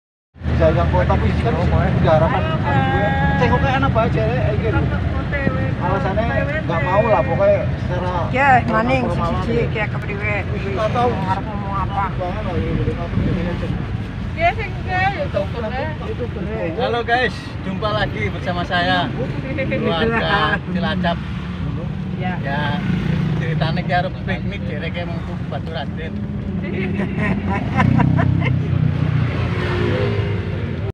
piknik nyewa angkot ke Baturraden Purwokerto
piknik bersama saudara, walaupun kita nyewa angkot 😂kita tetap bersyukur bisa berkumpul bersama. Bergembira tidak harus mahal persaudaraan itu yang harus kita jaga. Semoga selalu akur,sehat, selamat dunia akhirat 🤲